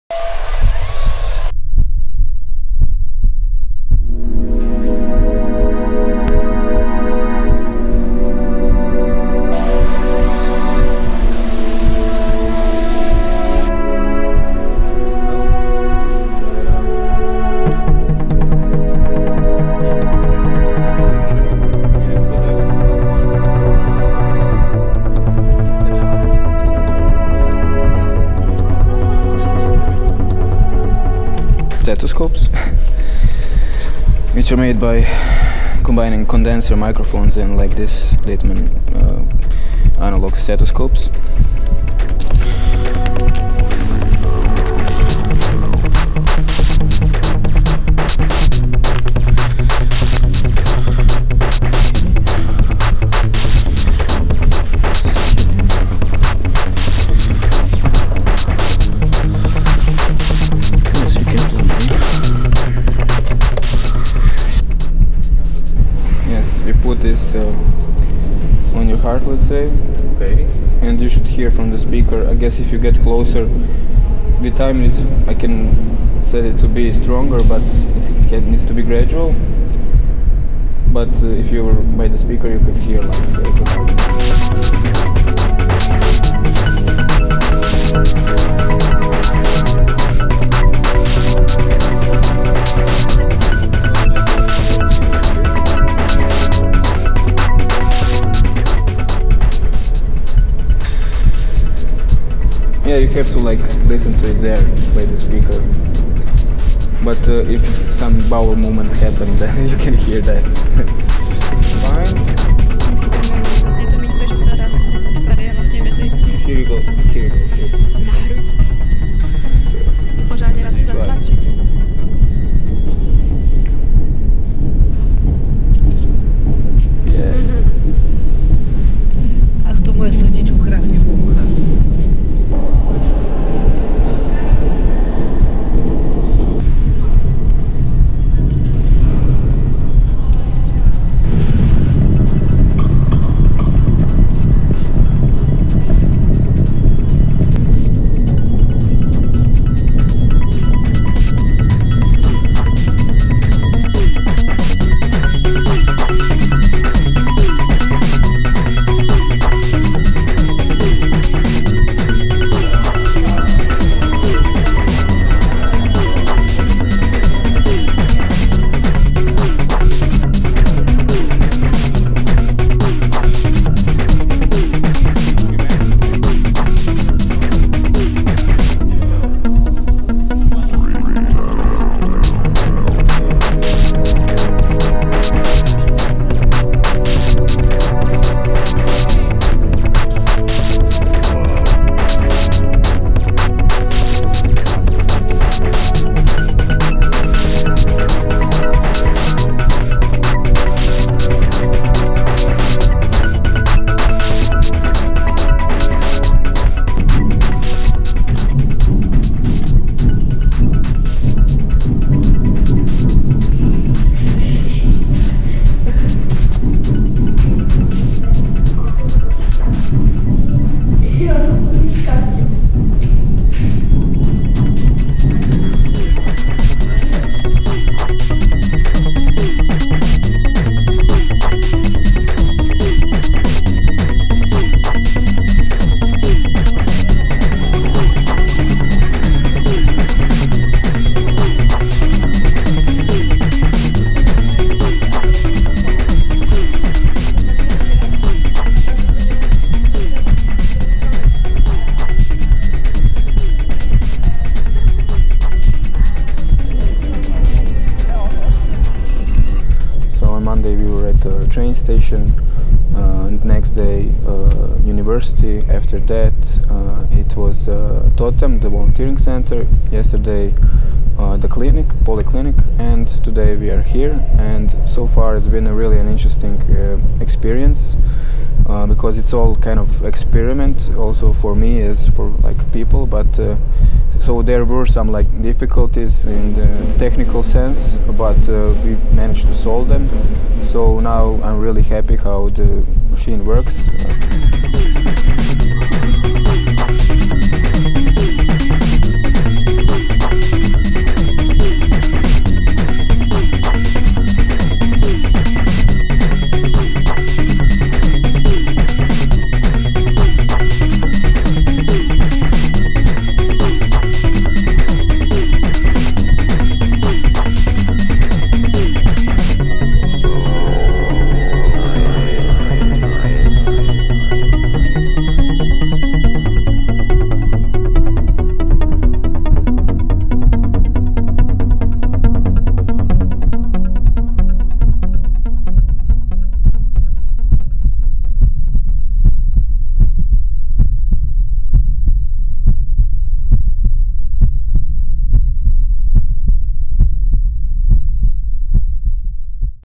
Sound of the body, Pilsen 2014
( if using a laptop listen with earphones, because of low frequencies )

Interactive installation

In 2014 i went on a residency to Pilsen, Czech Republic.
For my project i created a device- installation that projects body sounds ( heart, internal organs) of the users through speakers to the space of selected public buildings in real time. It consists of four electronic stethoscopes of my own design that are connected to a suitcase with audio components, a computer, and finally to speakers. The installation is easily transported by two people, and can easily be assembled, making it highly mobile.
It was set up over a course of one week in the following public buildings in Pilsen:

Monday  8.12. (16:20 - 18:20) Železniční stanice Plzeň - Jižní Předměstí ( train station)
Tuesday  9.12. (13:30 - 15:30)  Faculty of Electrical Engineering
Wednesday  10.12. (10:00 - 12:00) TOTEM - RDC (volunteering centre)
Thursday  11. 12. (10:00 - 12:00) Poliklinika Denisovo nábřeží ( polyclinic )
Friday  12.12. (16:00 - 18:00)  Cathedral of St. Bartholomew
Sunday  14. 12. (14:00 - 16:00) OC Olympia ( shopping centre)

People that occupied the spaces at fore mentioned times could try out the installation freely. Up to 4 people can use the device at the same time, creating a unified rhythm with their heart beat. I was interested in how it would feel to hear your heartbeat in a public space, surrounded by people that can hear it too, like a non verbal communication experiment. Each building would give  its own context and „ feel“ combined with the sound of the beating heart.
The base concept is transmission of internal organs sounds of people to the space of the public building, which is an organ of the city.